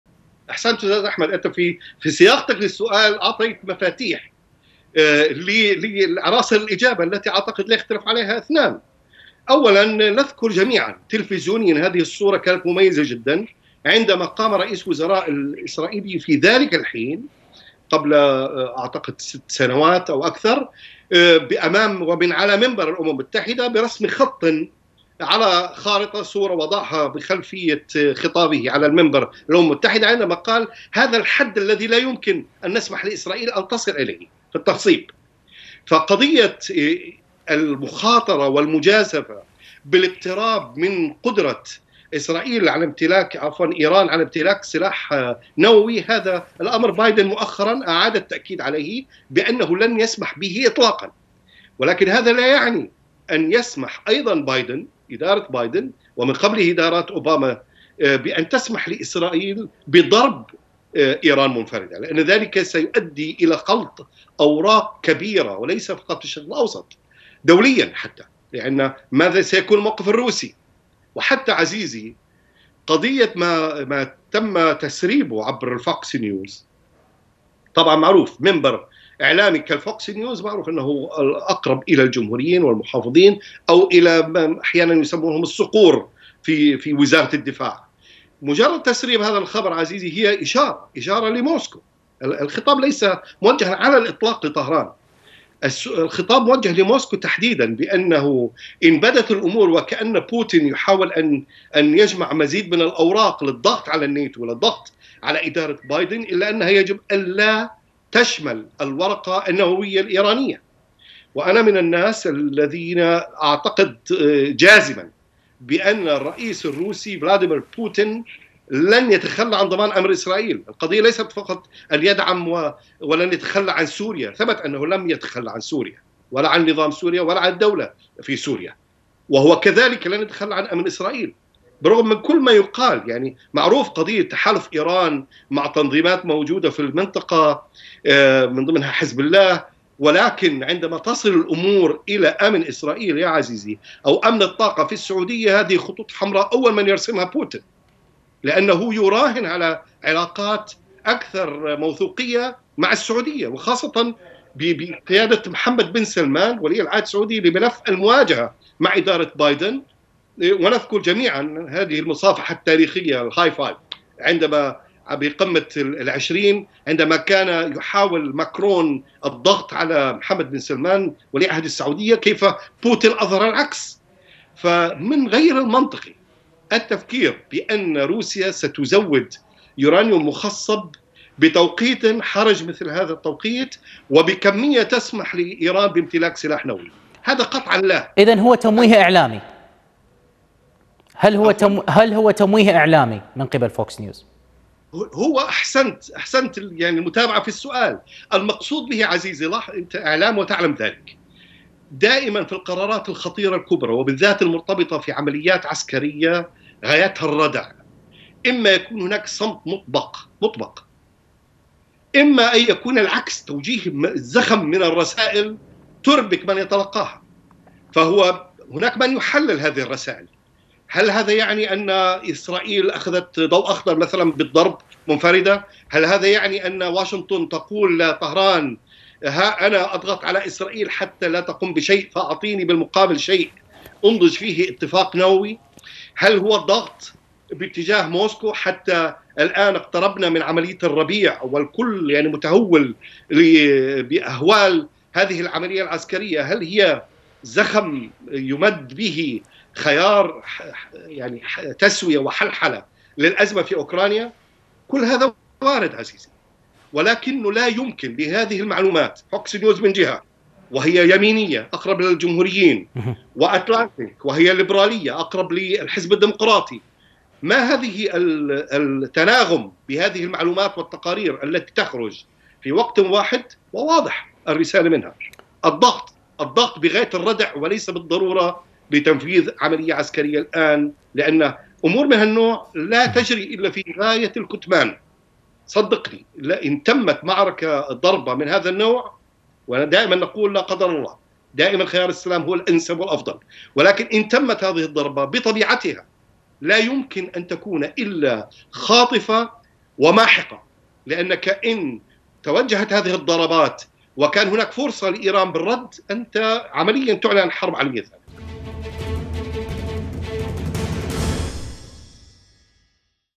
جرار : بايدن لن يسمح لـ اسرائيل بضرب ايران منفردة
موقع قناة التغيير الفضائية  الالكتروني